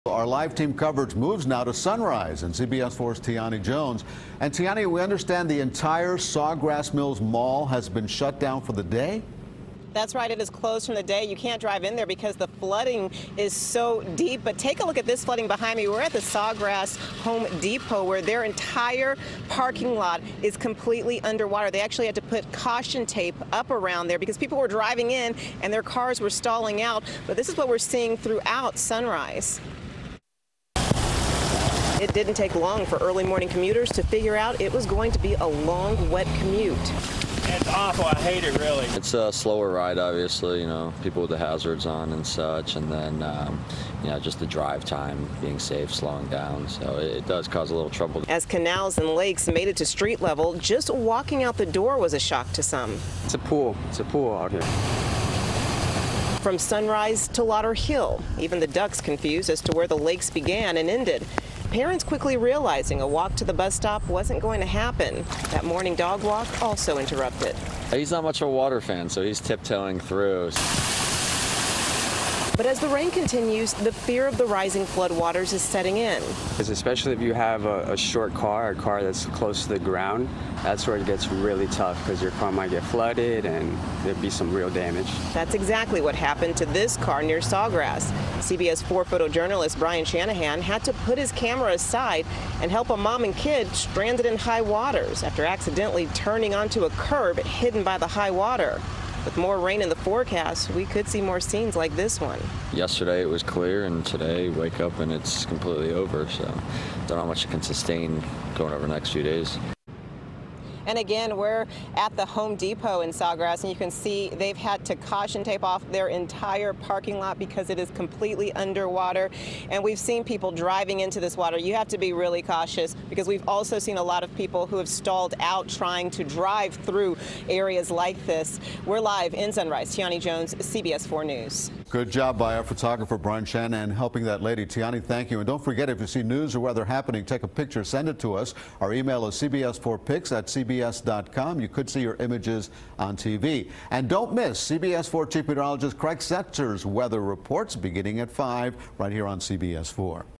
Caution Advised For Drivers Around Sawgrass Mall In Sunrise
CBS Miami's Tiani Jones reports from Sunrise.